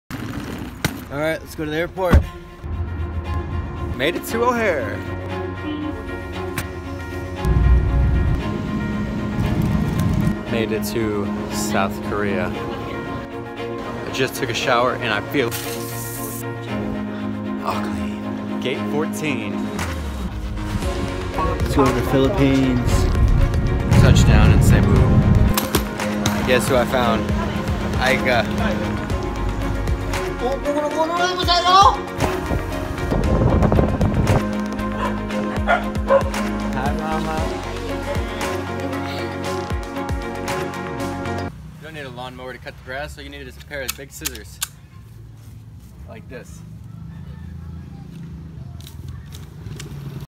30 hours in 39 seconds
chicago to daanbantayan, cebu, philippines via 4 modes of transportation.